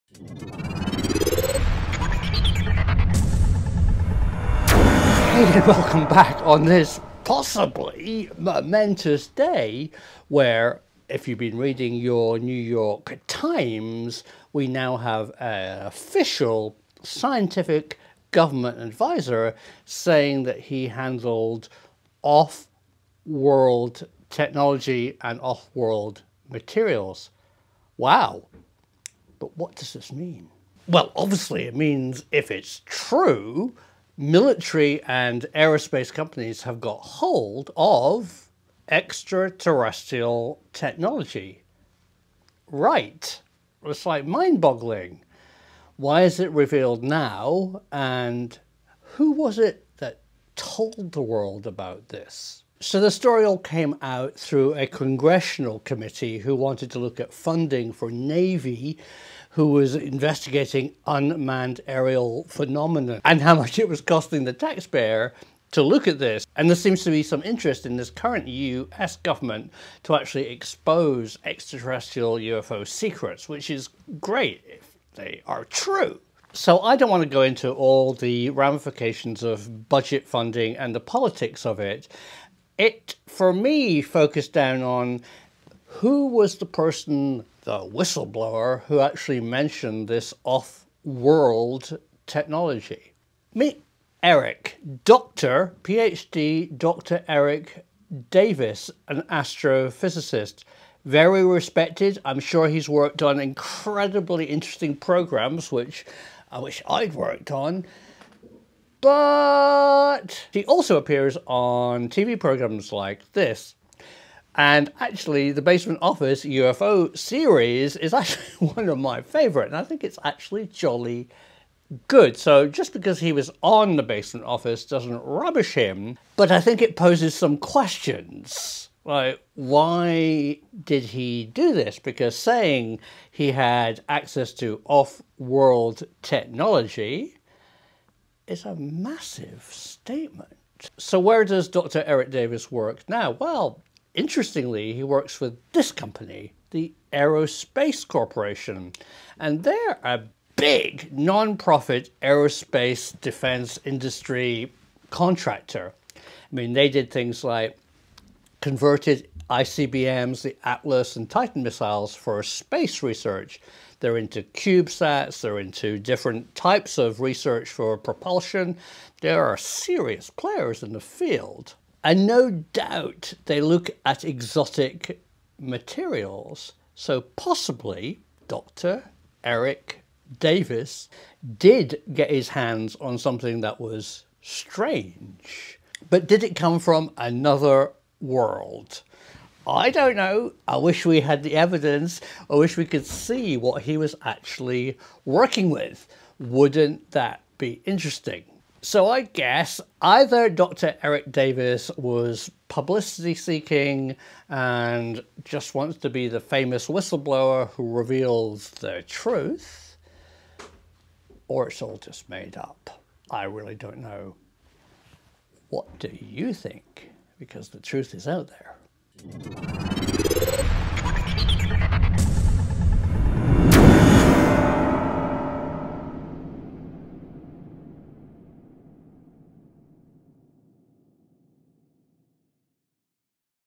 'Off world technology with Dr Eric W Davis'
A US gov contractor, claims to have worked with 'off world technology' and alien materials.  Prof Simon investigates.